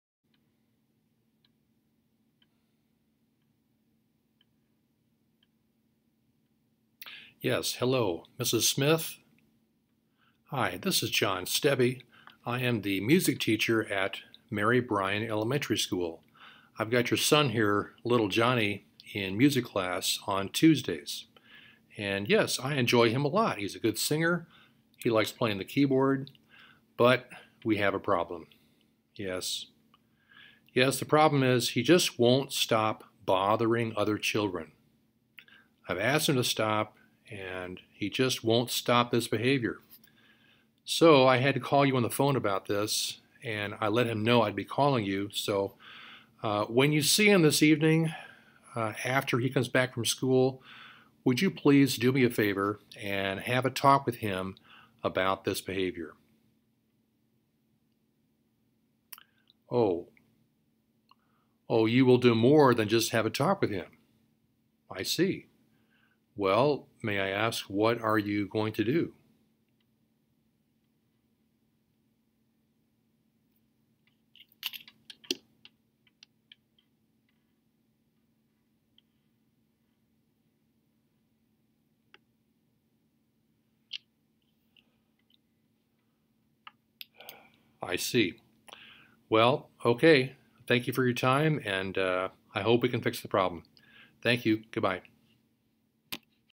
Phone call home
This is a "cautionary tale" I show to kids on the first day of music class.